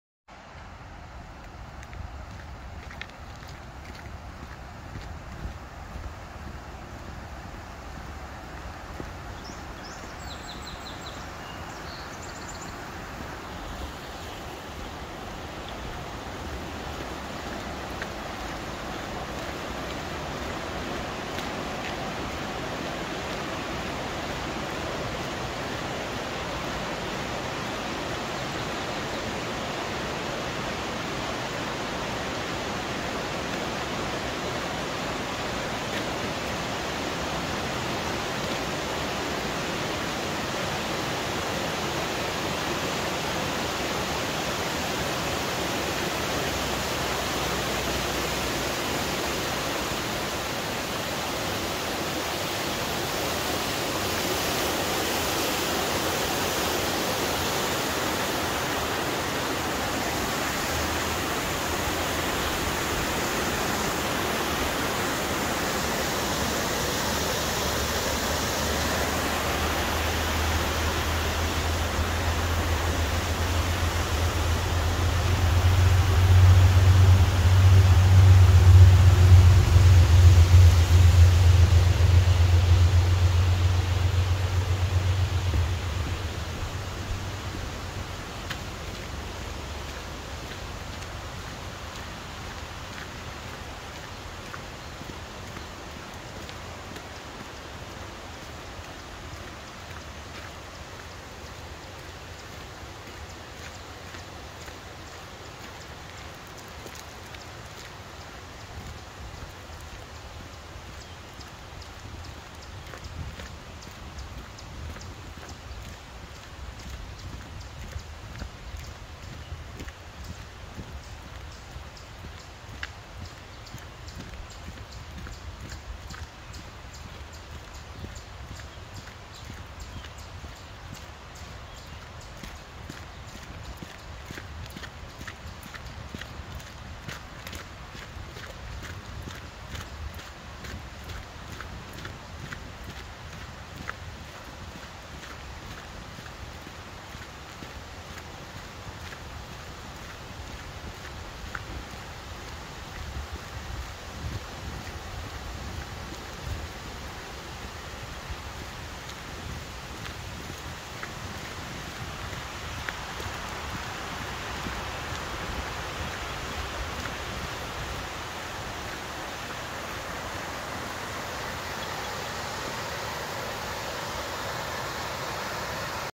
Morning walk at Corbett's Glen Nature Park - just outside Penfield (3 minutes)
This 3-minute walk takes you to Corbett's Glen Nature Park - located just outside Penfield. The sun shines brightly in the morning and the water creates beautiful reflections. Enjoy the peace and greenery on this Spring morning.